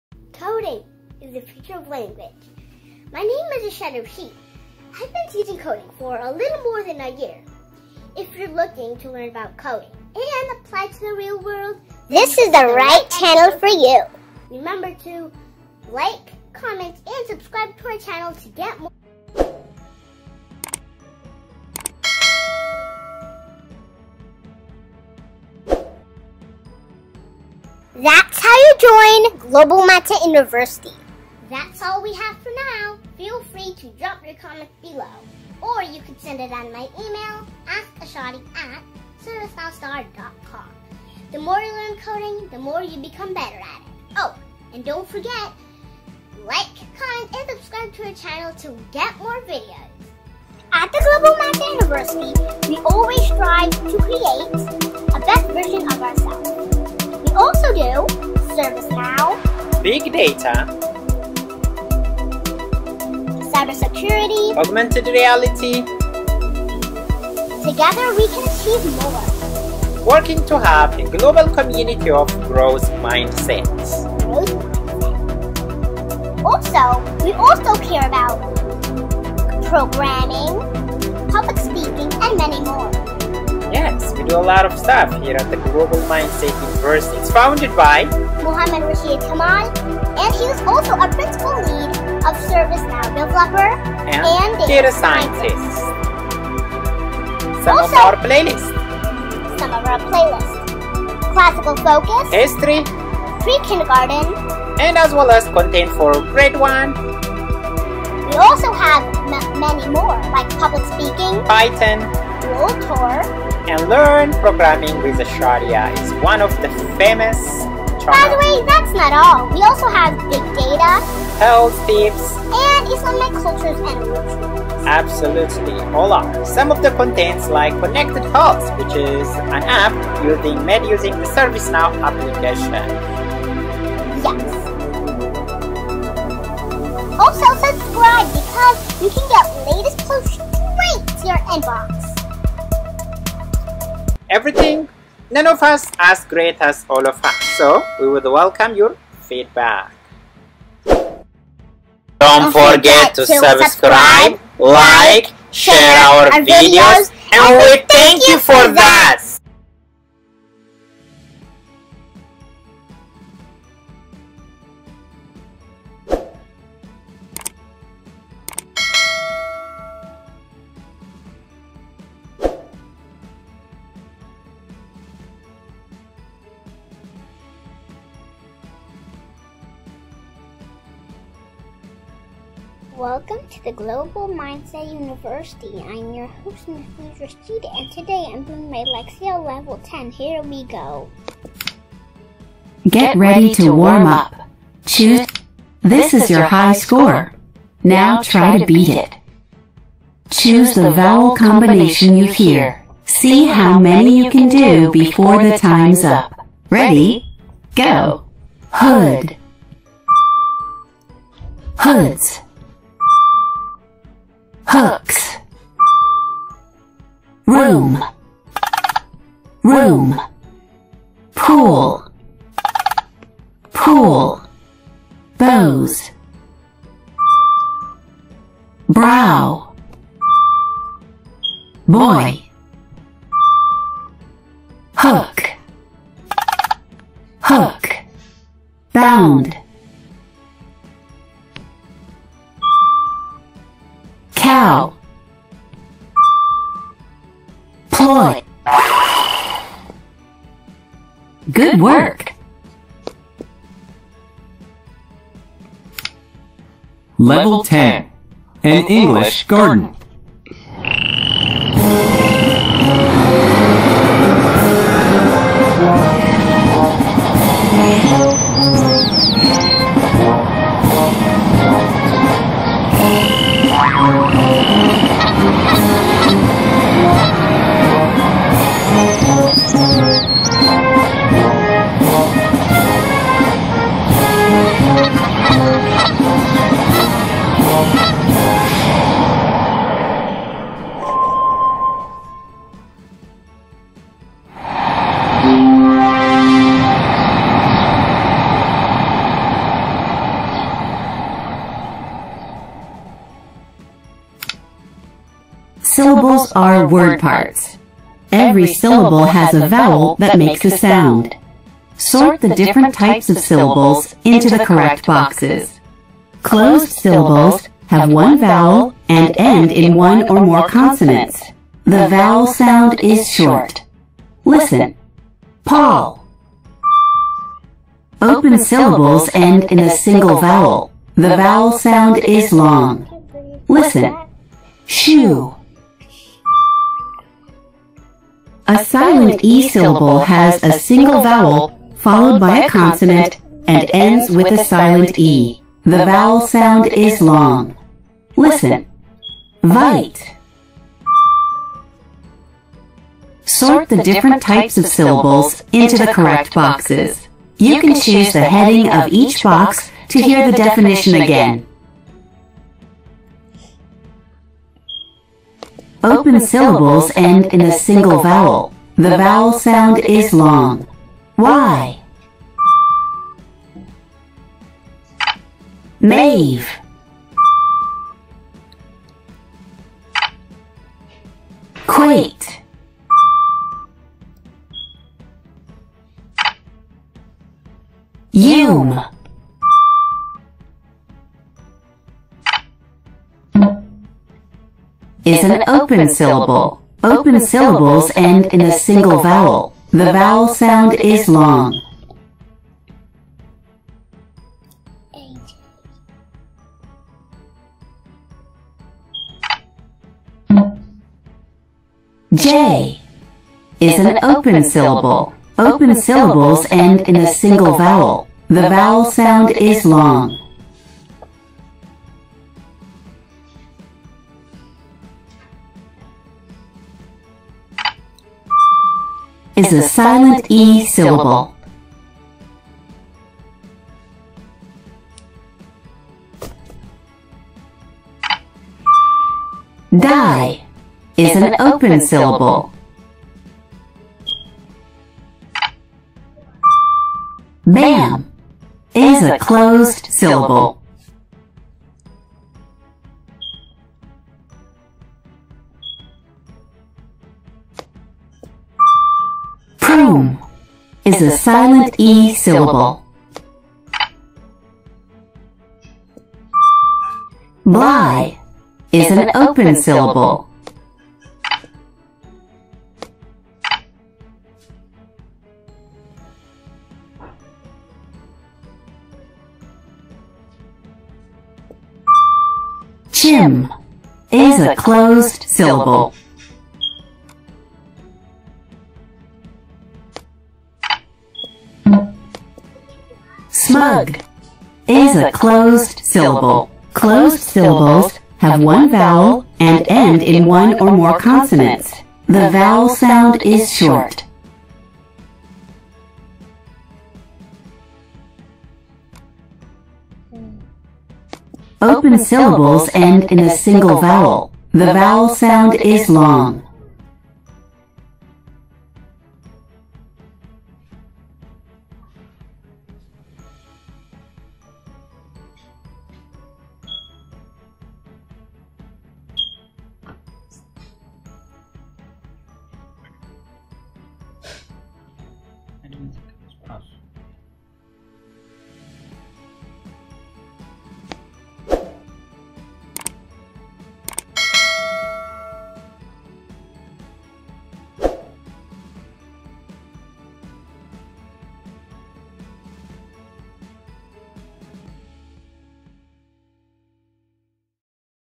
Lexia Core 5 level 10 | How to write Multi Syllable Word? Identifying multi syllable words
Viral video for kids reading improvement. This episode is about multi-syllable words.
Lexia Core5 is a learning platform providing differentiated literacy instruction for students of all abilities in grades pre-K–5.
Lexia Core 5 - Level 7 Completion!!